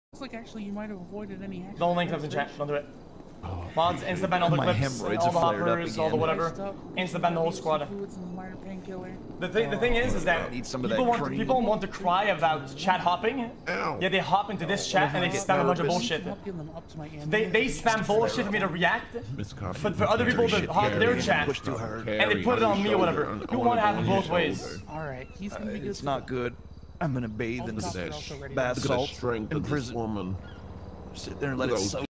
xQc with drastic Actions against Chat Hoppers (Everyone got banned)
Support the Streamer in this Clip!!